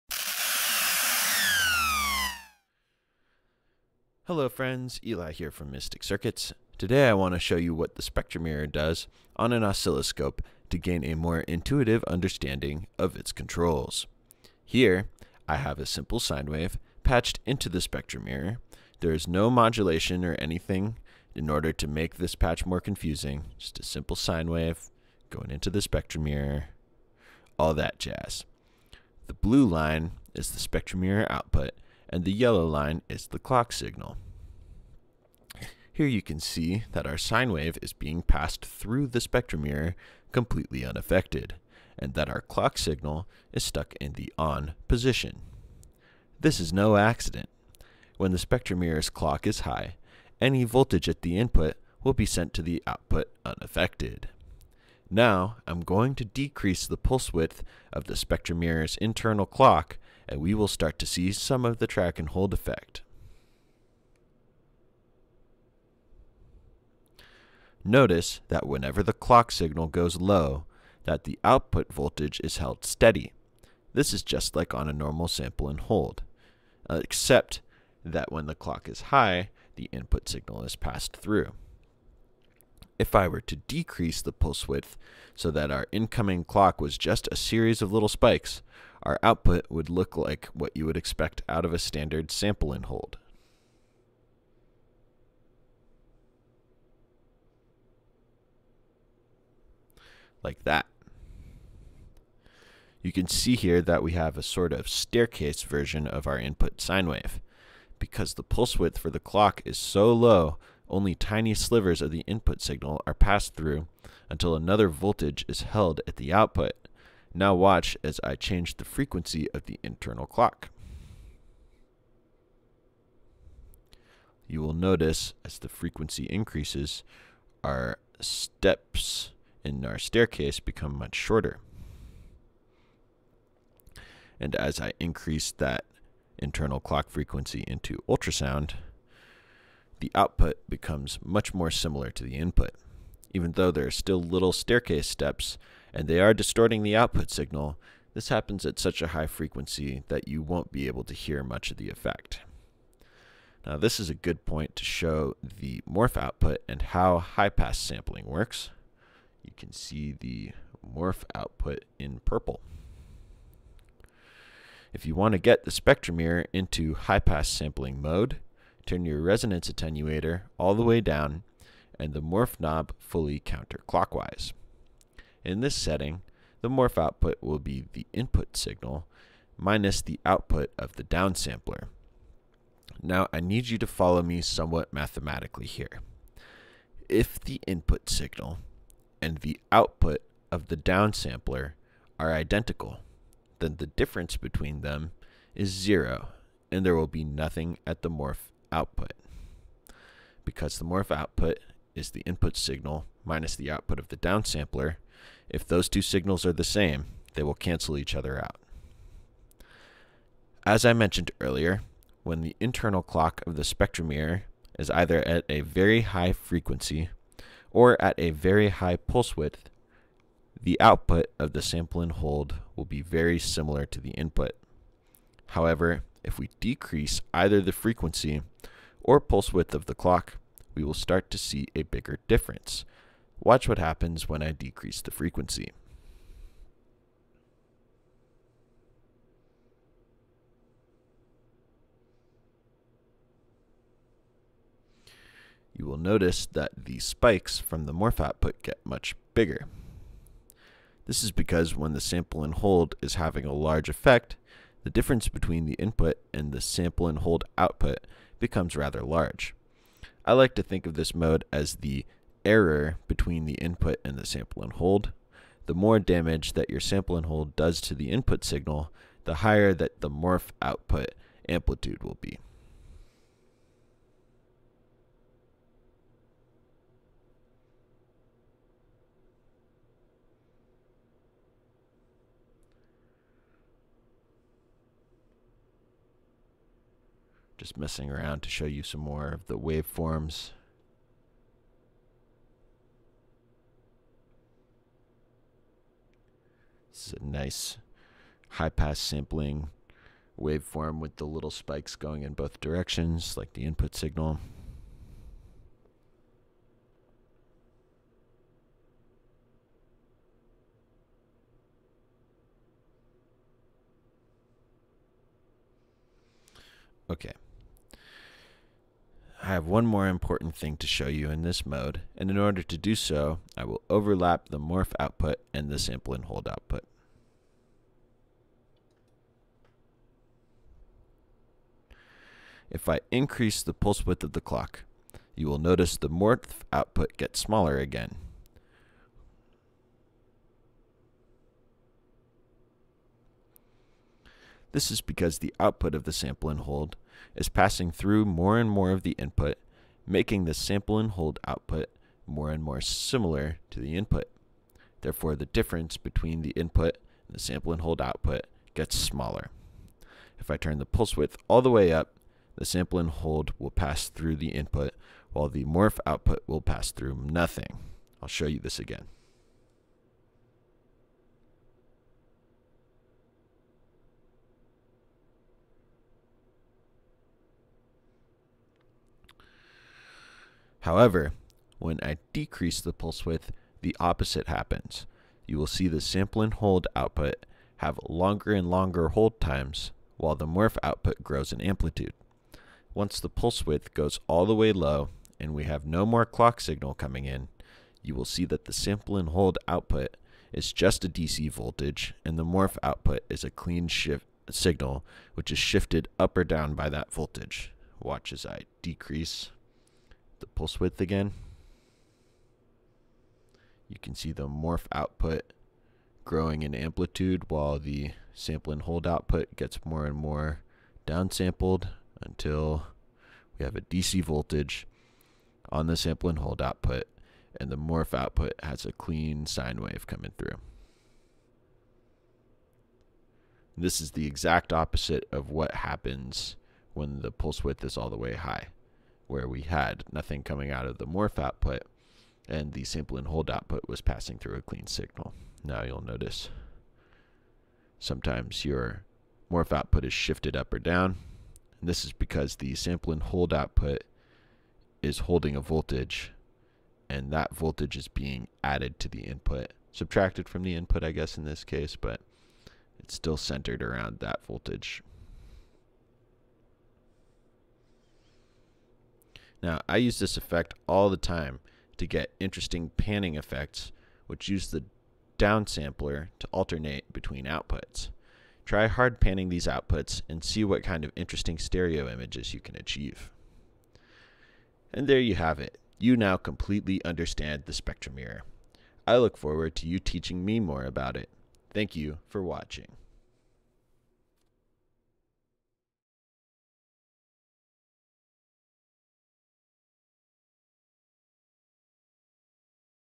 Spectra Mirror Oscilloscope Explanation
This is probably the best way to show exactly what the Spectra Mirror does to a signal.  I briefly explain Track and Holds and how they differ from Sample and Holds, "High-Pass" Sampling and how to use it in conjunction with the normal Spectra Mirror effect.